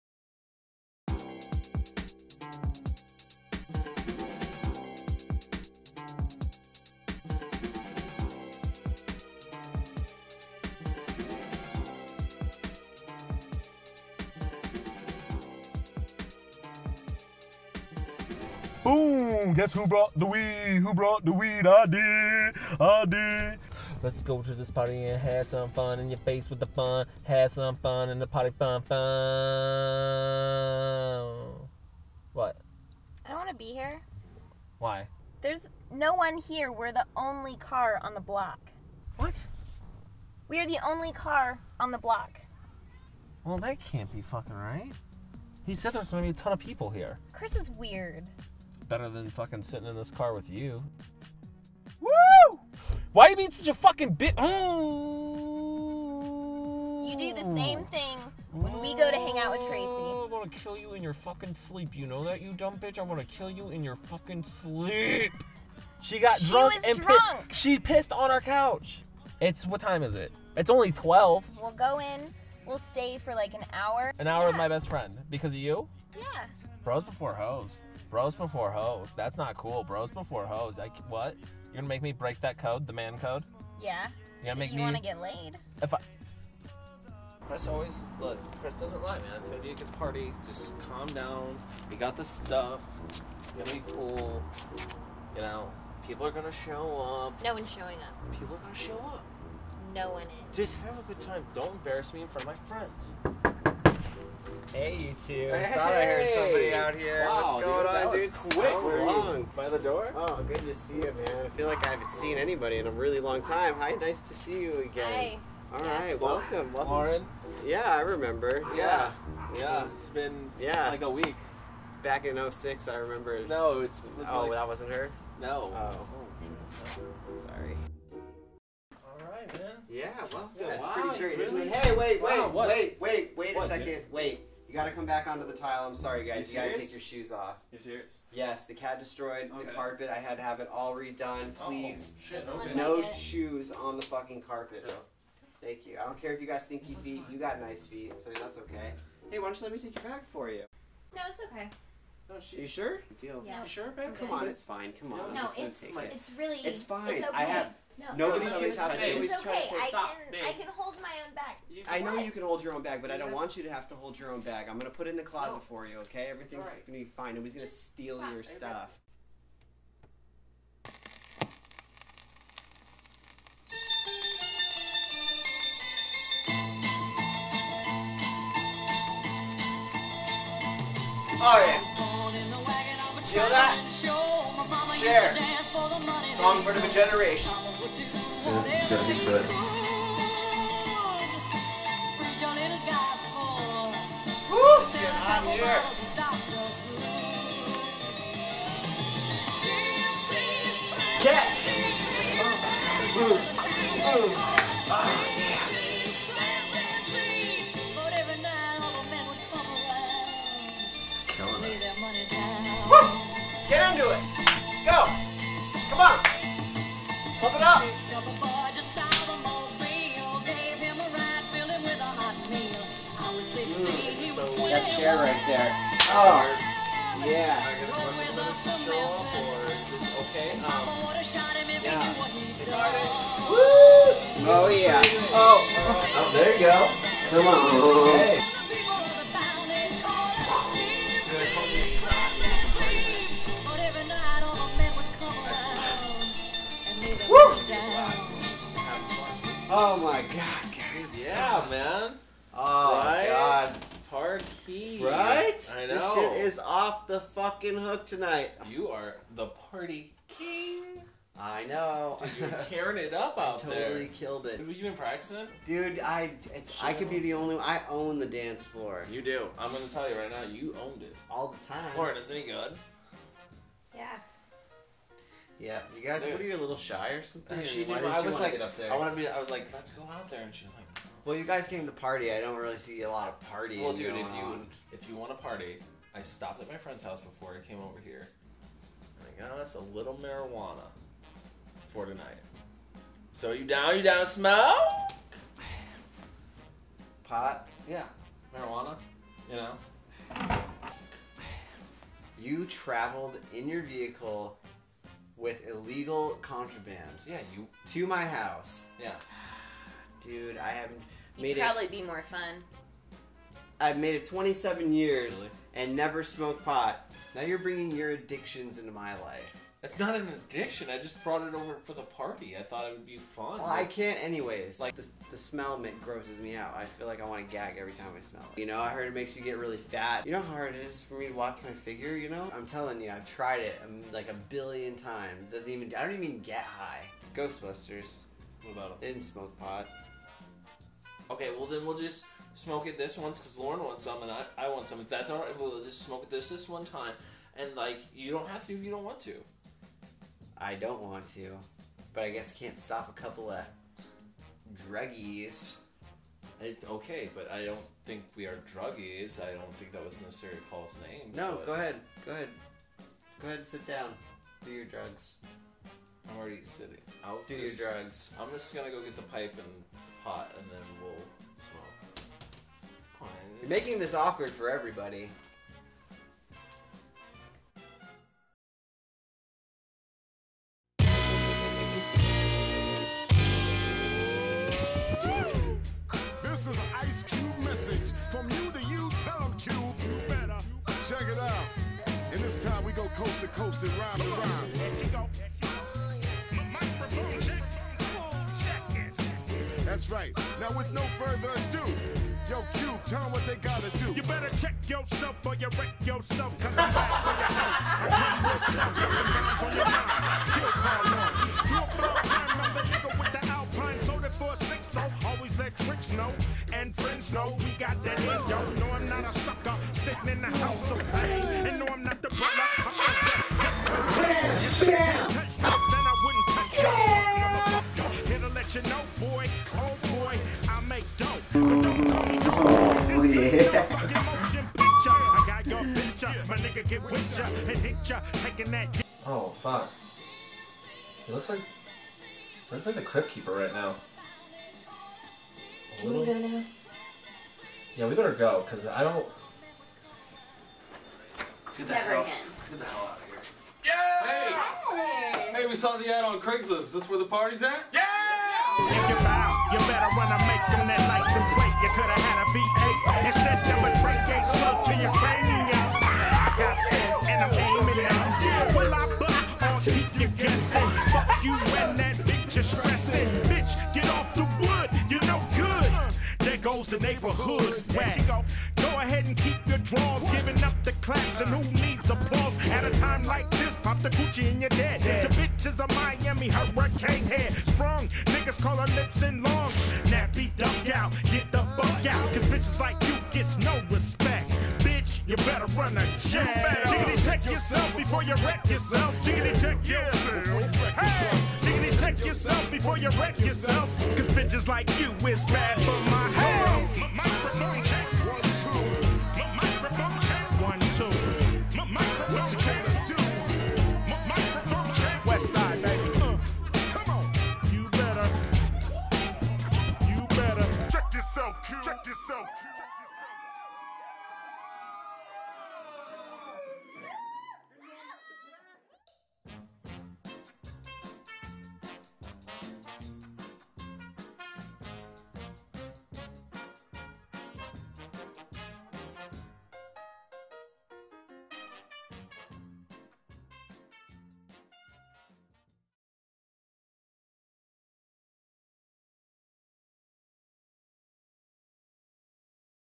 The Party
A Short improvised film from the people who brought you Peanut Butter. A couple shows up at a friends house expecting to do some serious partying. Instead, they're the ones trying to create it by raising the stakes.